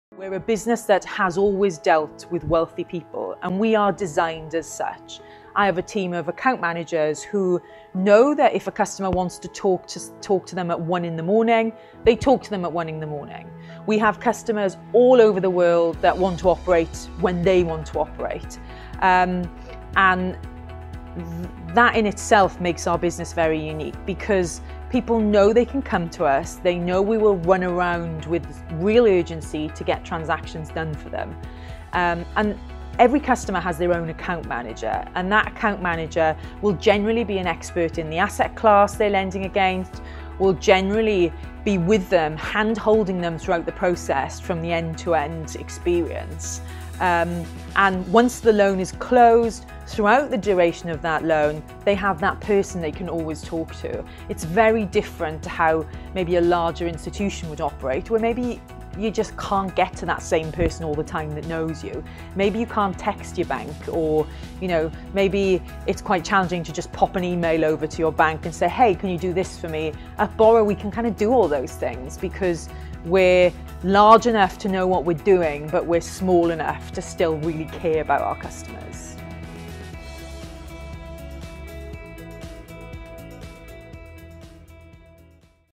How Borro Provides Superior Customer Service
Borro's Executive Vice President of Global Originations, Claire Hillier, describes the company's client base and how their needs are met.